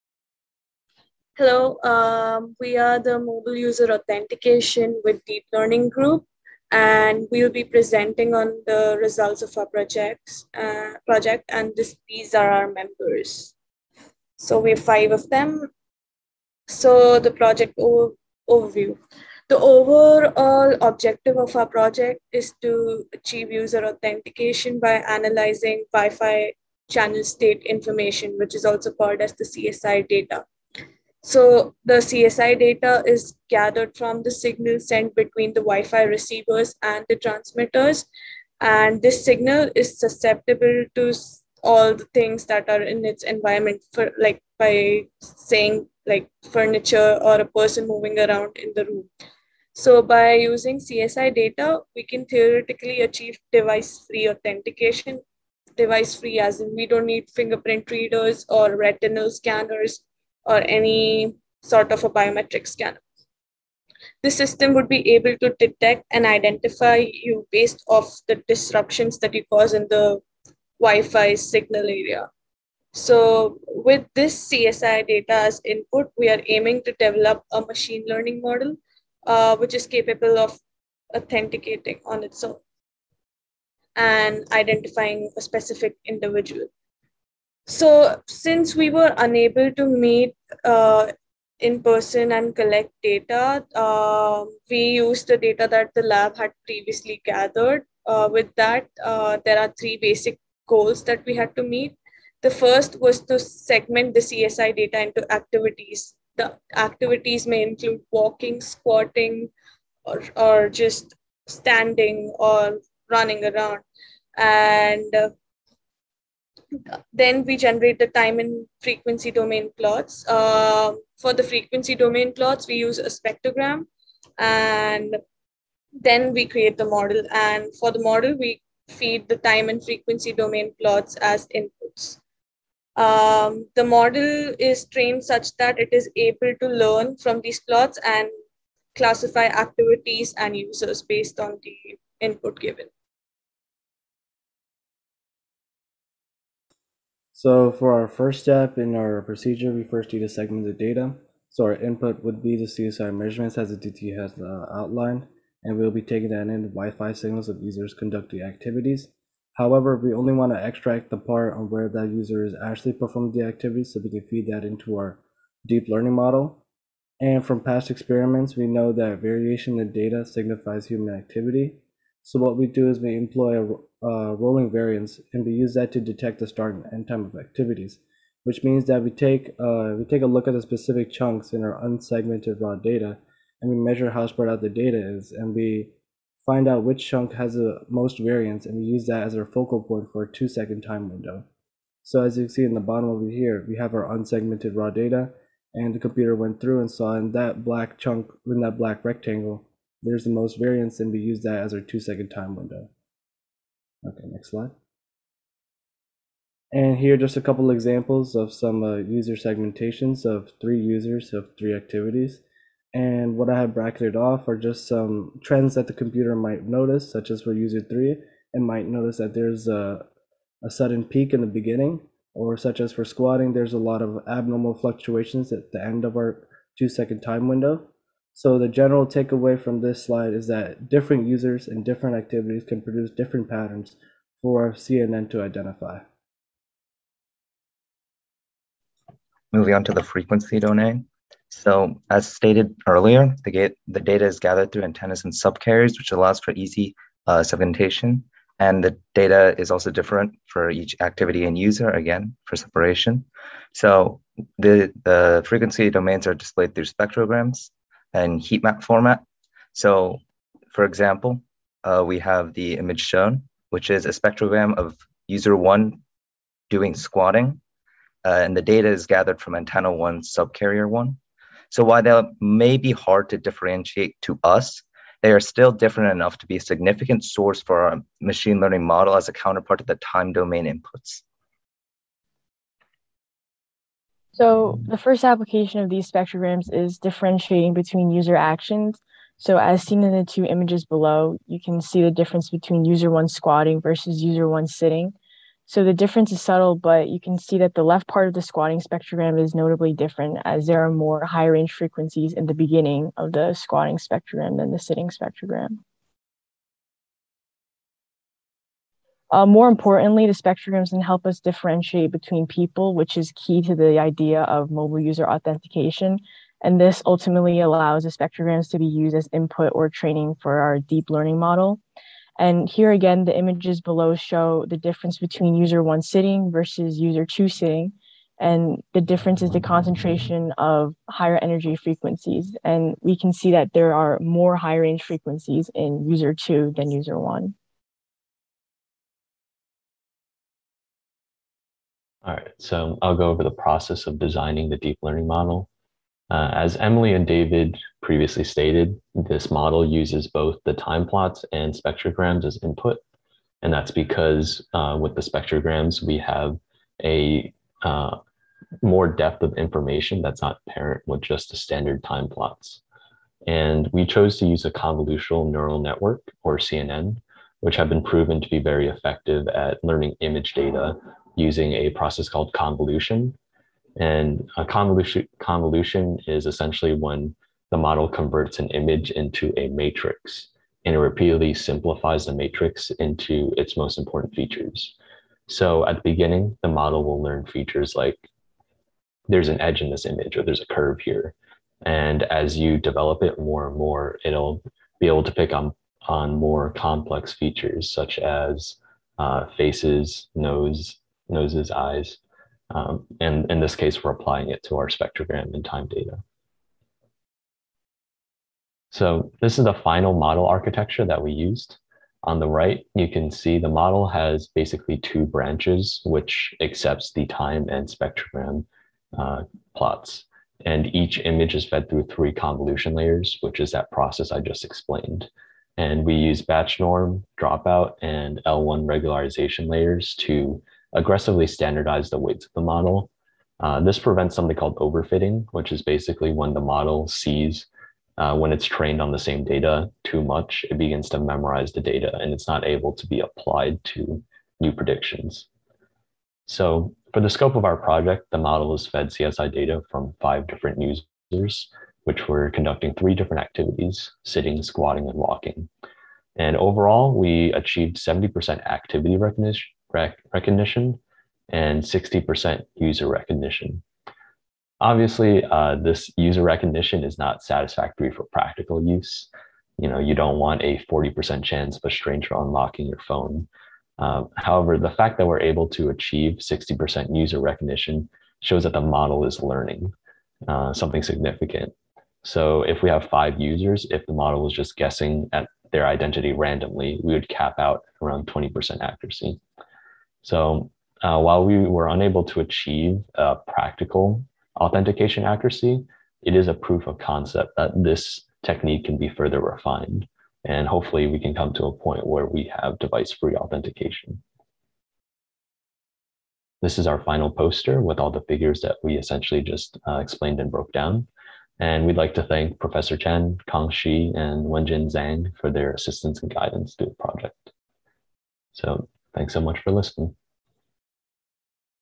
2021 Summer Internship Presentation: "Mobile User Authentication via Deep Learning"
Title: "Mobile User Authentication via Deep Learning"

Team Members:
Emily Gao
Daniel Liu
Sharad Prasad
David Man
Aditi Satish